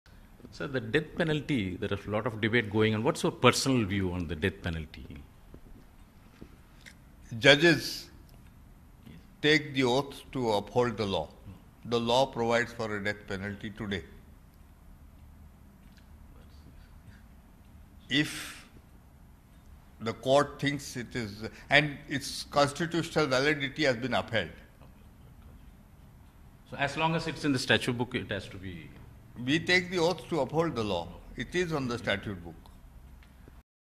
"Took Oath To Uphold Law, Law Prescribes Death Penalty": Chief Justice-Designate SA Bobde
Justice SA Bobde, who will take over as the country's top judge in November, today said that he has no objection to the death penalty so long as the law provides for it. "Judges take an oath to uphold the law. The law provides for the death penalty today, and its constitutional validity has been upheld. It is in the statute book," he told NDTV in an exclusive interview that covered various facets of the judiciary's functioning.